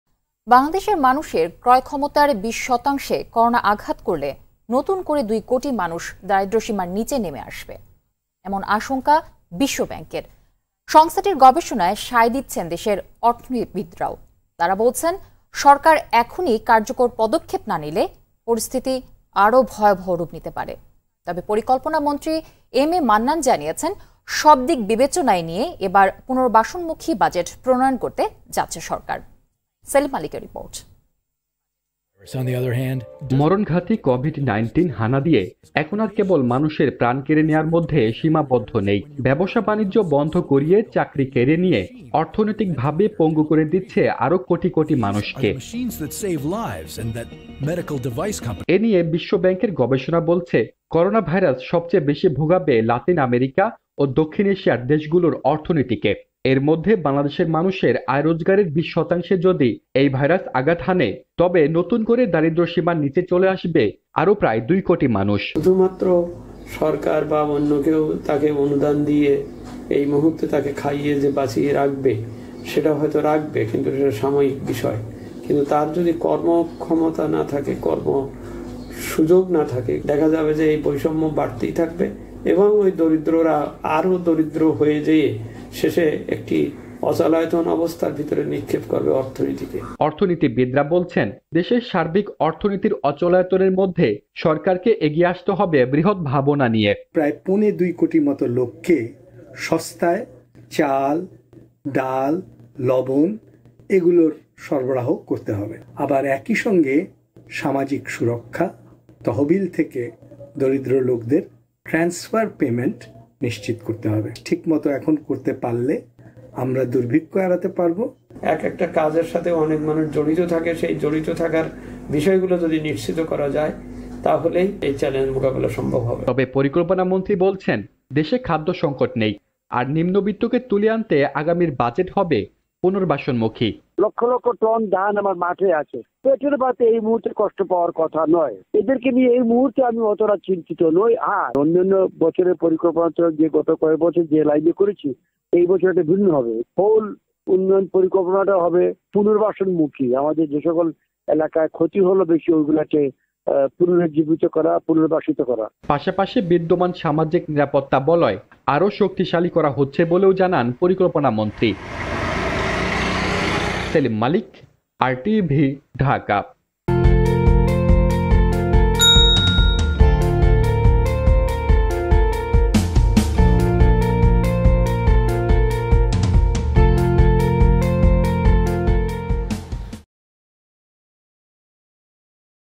. করোনা আঘাতে আরো দুই কোটি মানুষের দারিদ্রসীমার নিচে নেমে যাওয়ার আশঙ্কা বিশ্বব্যাংকের।
News : . করোনা আঘাতে আরো দুই কোটি মানুষের দারিদ্রসীমার নিচে নেমে যাওয়ার আশঙ্কা বিশ্বব্যাংকের।

- Team RTV